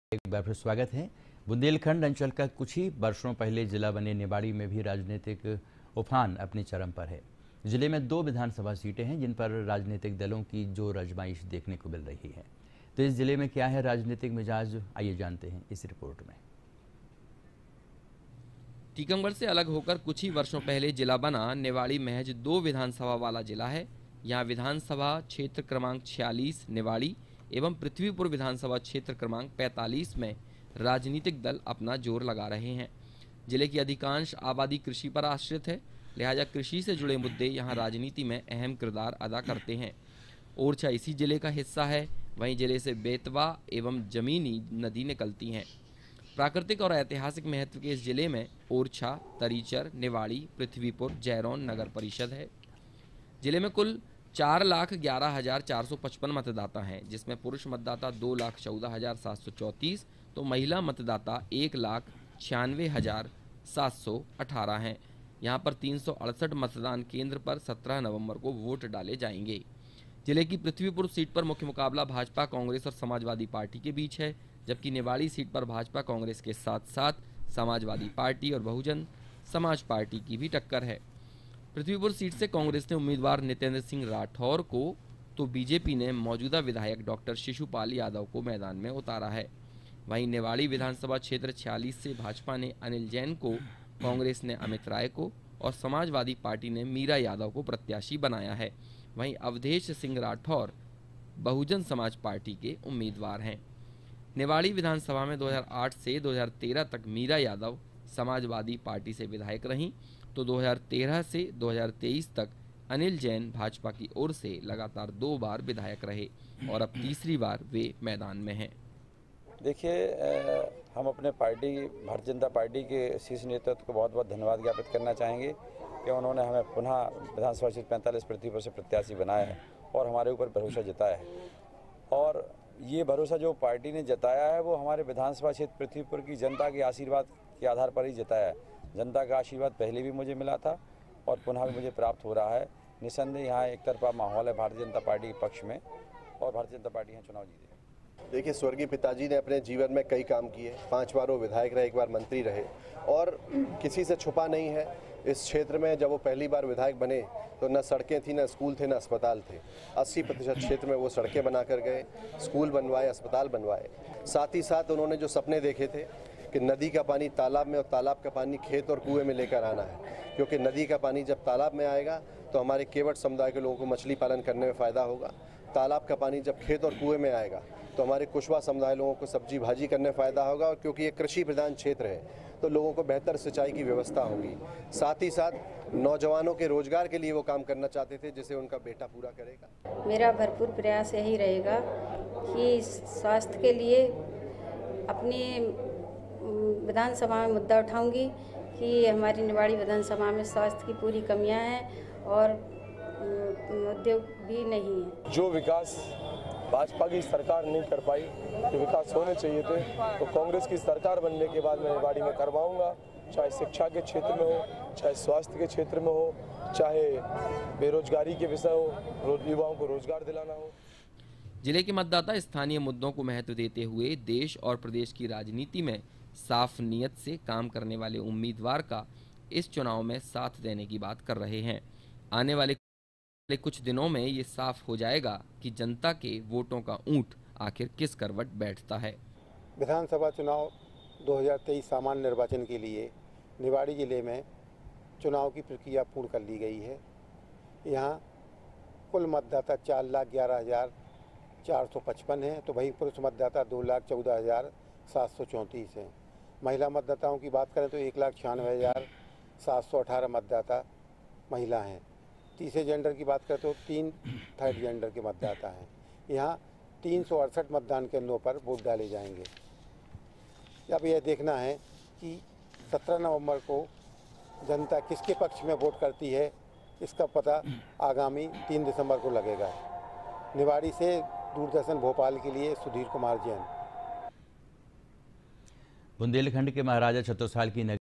निवाड़ी जिले का विधानसभा प्रोफाल ,निवाड़ी 46 एवं पृथ्बीपुर 45 का
पृथ्बीपुर ,निवाड़ी जिले की विधानसभा चुनाव का मुकावला ।